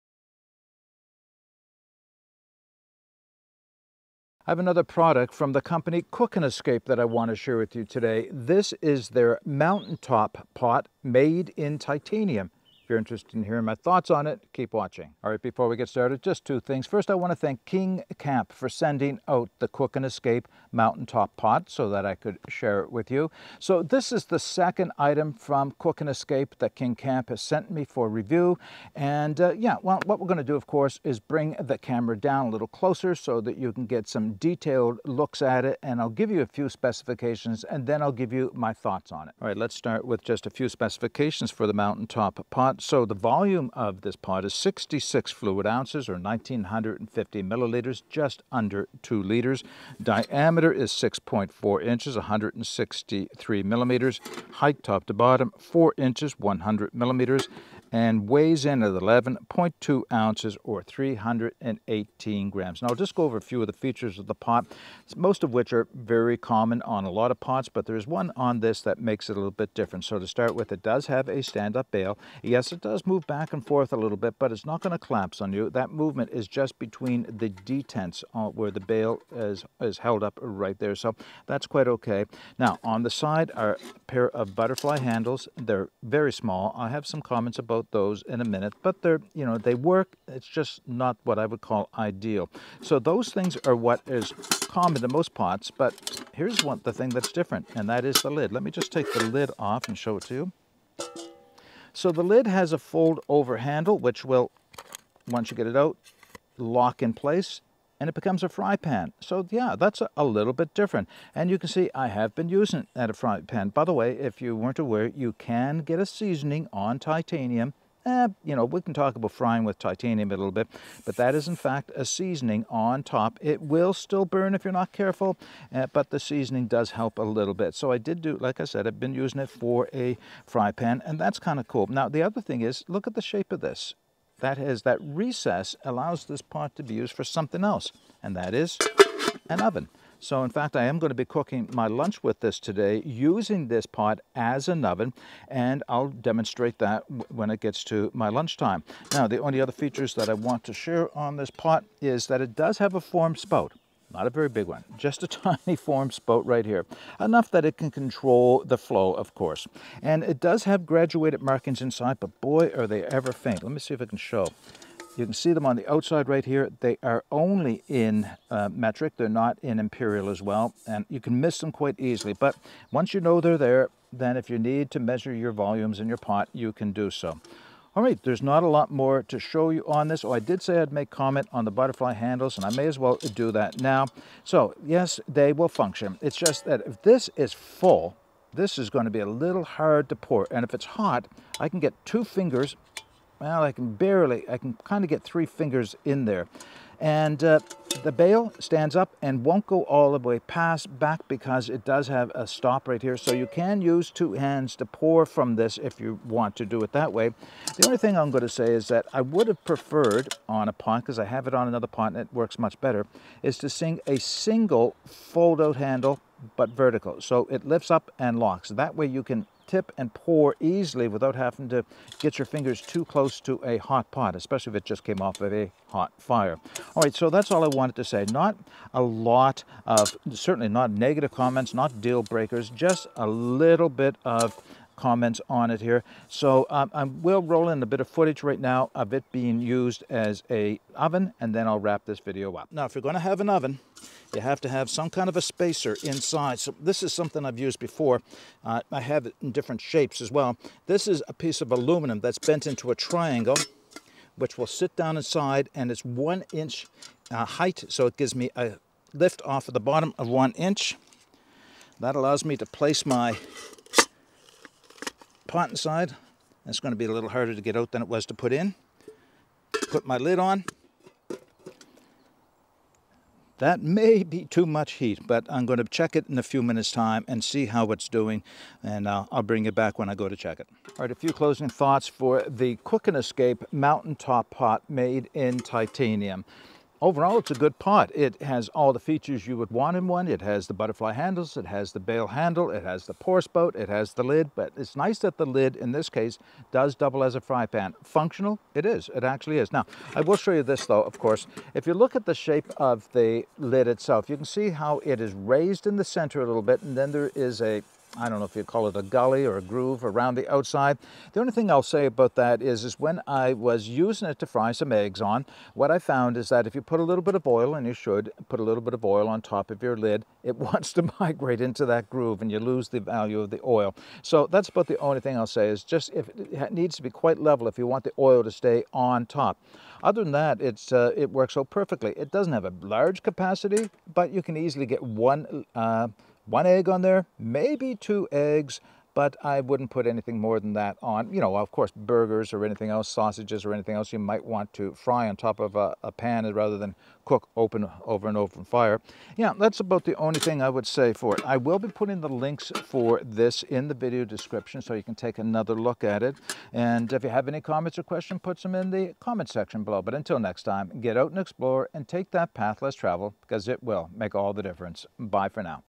Cook N Escape Mountain Top Pot
$59.99USD
Volume  66oz / 1950ml
Diameter  6.4” 163mm

Post has a small pour spout
Can be used as an oven
Lid can be used as a frying pan (rudimentary seasoning)
Butterfly handles are too small. Would be better if they were a lift ring
Bail is secure but feels looses as it moves inside the detents